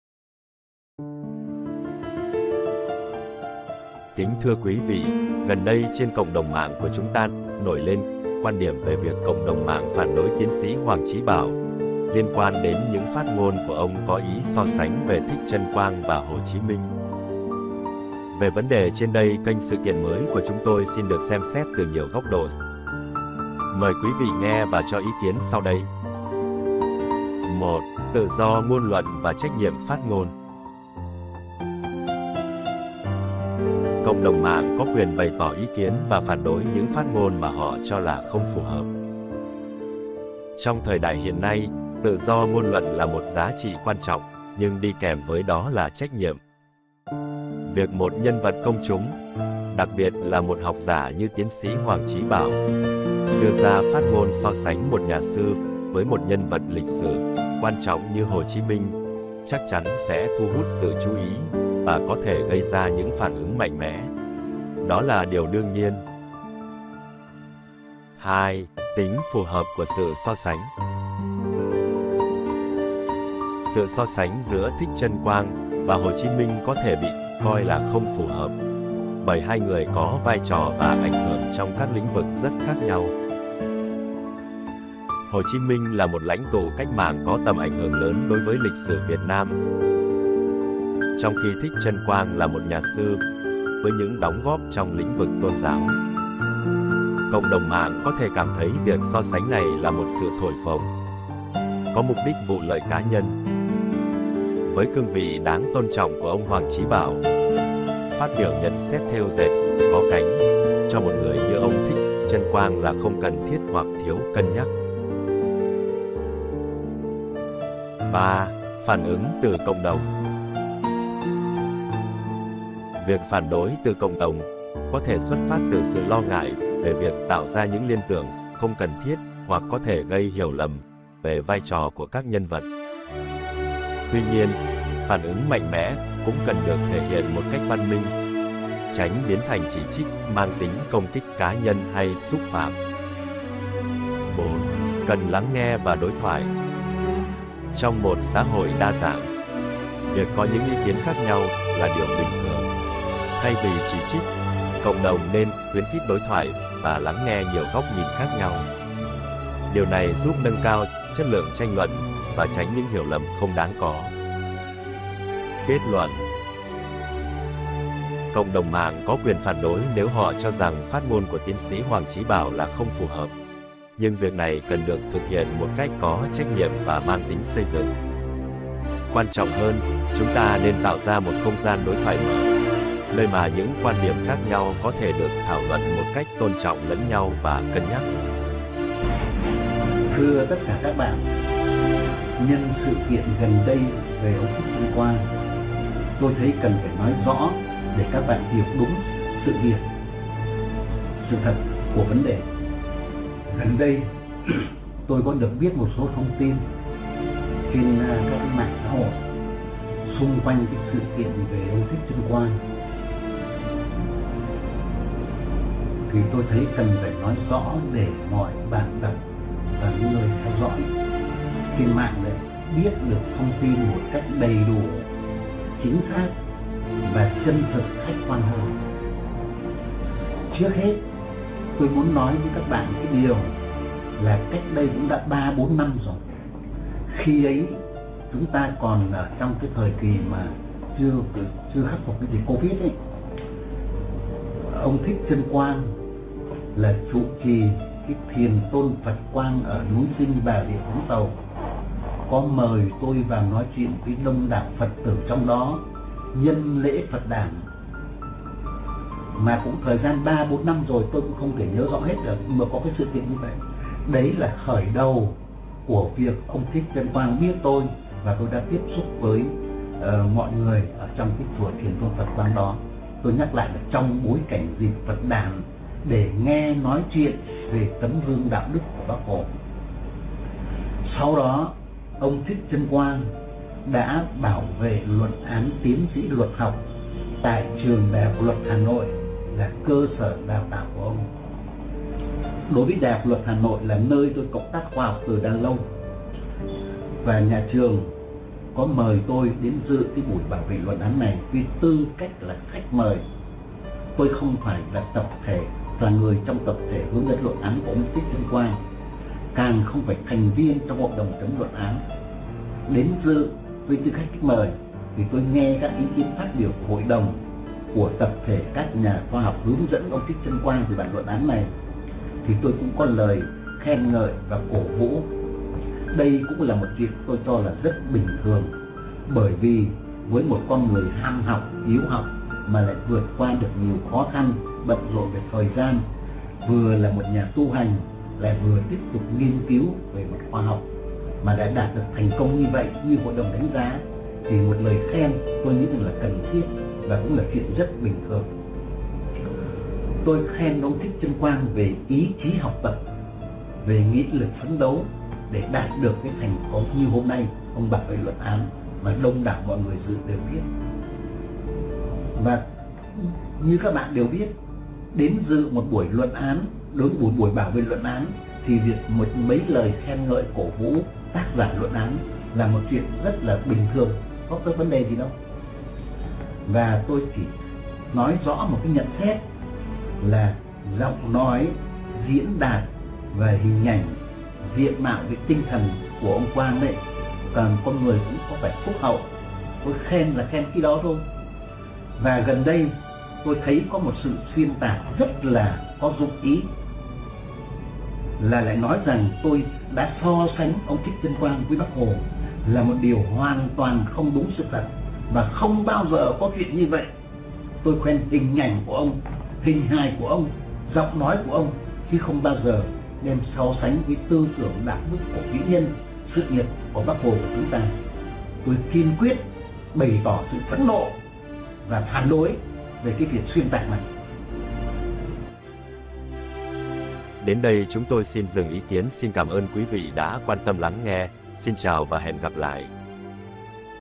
Cộng Đồng Dậy Sóng: Phát Ngôn Của Hoàng Chí Bảo
Video này thảo luận về phản ứng của cộng đồng mạng đối với phát ngôn gây tranh cãi của Tiến sĩ Hoàng Chí Bảo khi so sánh Thầy Thích Chân Quang với Hồ Chí Minh. Những lời phát biểu này đã làm dấy lên nhiều luồng ý kiến trái chiều và thu hút sự chú ý rộng rãi trên các nền tảng truyền thông xã hội.
Chúng tôi sẽ phân tích các quan điểm khác nhau, từ những người ủng hộ đến những người phản đối, đồng thời thảo luận về ý nghĩa và tác động của những phát ngôn này đối với xã hội Việt Nam. Video cũng đề cập đến vai trò của tự do ngôn luận và trách nhiệm trong việc phát ngôn, đặc biệt là khi những phát ngôn đó liên quan đến các nhân vật lịch sử quan trọng.
Hãy tham gia cùng chúng tôi trong cuộc thảo luận này và chia sẻ suy nghĩ của bạn trong phần bình luận bên dưới.